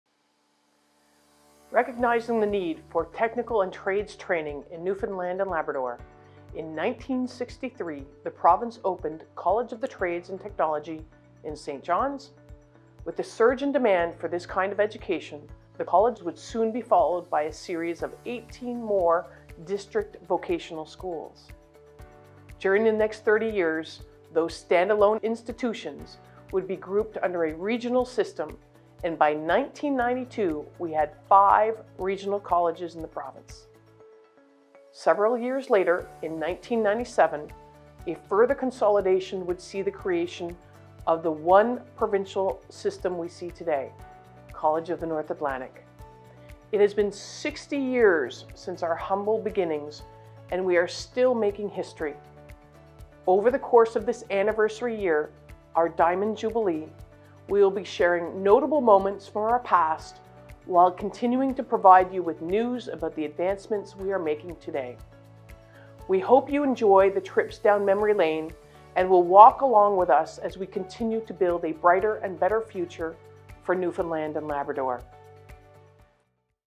CNA's 60 Anniversary!
It's time to celebrate! CNA is turning 60 and we are looking back at the incredible history of our esteemed educational institution. During the time of Beatlemania and MLK's famous "I Have a Dream" speech, the Newfoundland government introduced an innovative post-secondary education system and we've been soaring ever since. Join us over the next year in honoring the past 60 years of success and looking to the years to come!